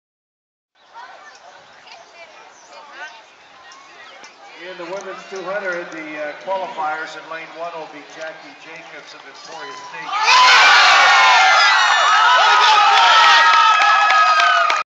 Brett Ottley Fort Hays State
MIAA Outdoor Championships - Men's Pole Vault